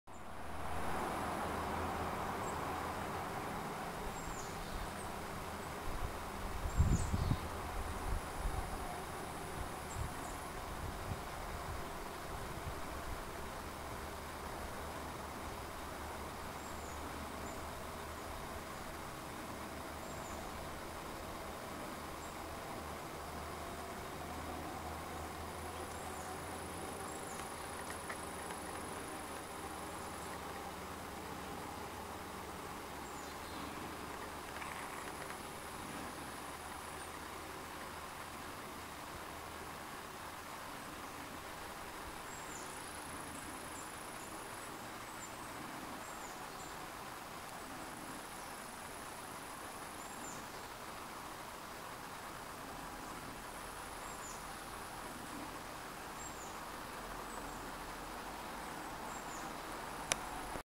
動画20181015「秋の平庭」＠岩手県久慈市13:35
朝晩の気温が一桁台になってきたので、出かけてみました。
一部赤く色ずく葉っぱが見えていました。